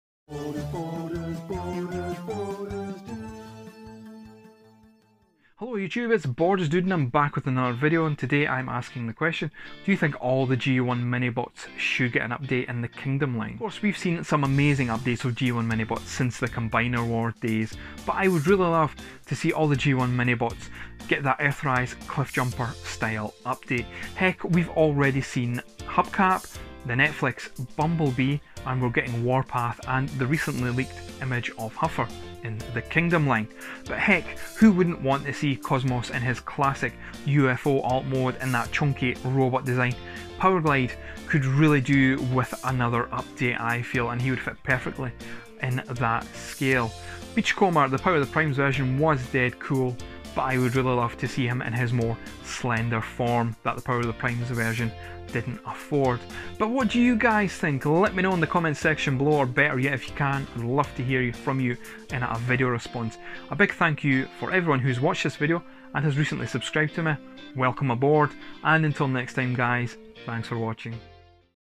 Should All The G1 Minibots Get The War For Cyberton Update Treatment ?!??!
Should all the G1 minibots get updated in the style of earthrise Cliffjumper?